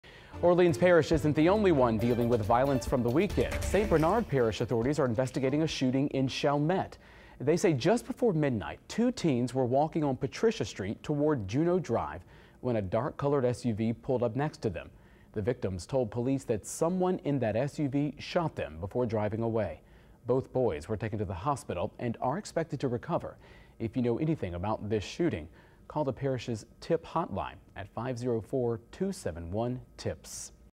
Two teenagers were shot in Chalmette
Two teenagers were shot while walking on a street in Chalmette after a car approached them.